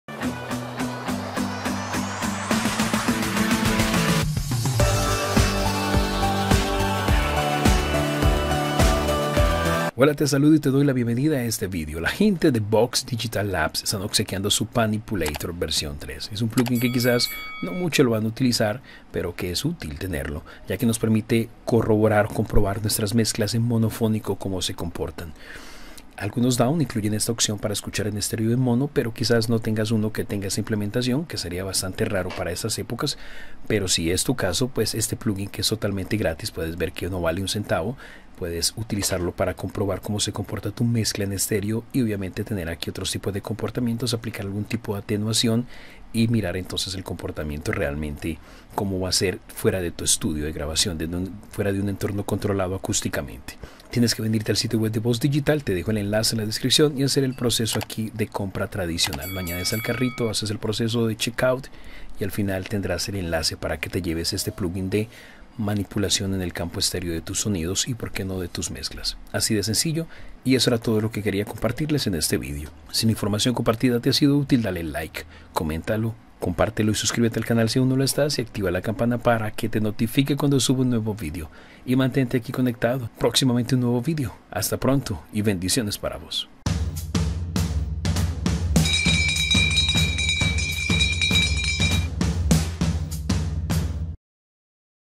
⚡Panipulator 3⚡¡GRATUITO!
Comprueba tus mezclas tambien en mono con este plugin gratuito.